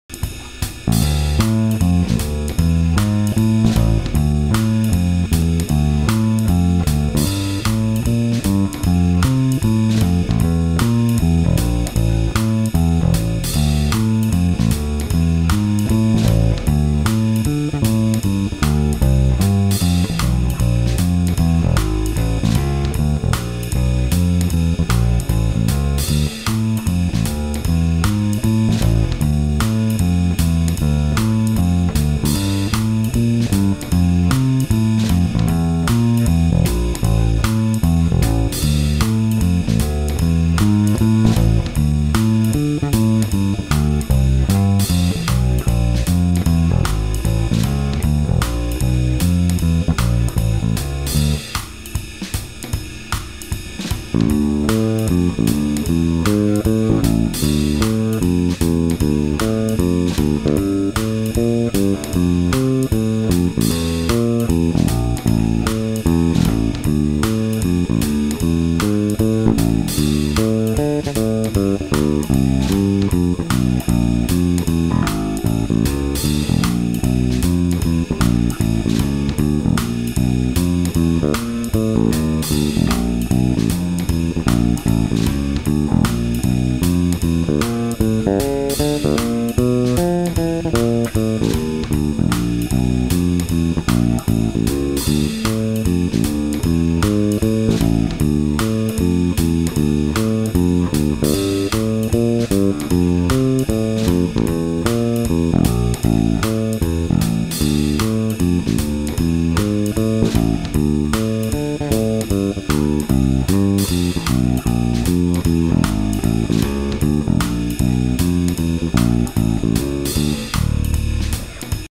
Dragonfly CJ5 D-Fly front and rear pickup test
Quick pickup test jam.
2020 Dragonfly CJ5 D-Fly
ash/Pau ferro
1 piece maple neck
Bass came from Danny Stewart at Bass Japan Direct. Both highly recommended.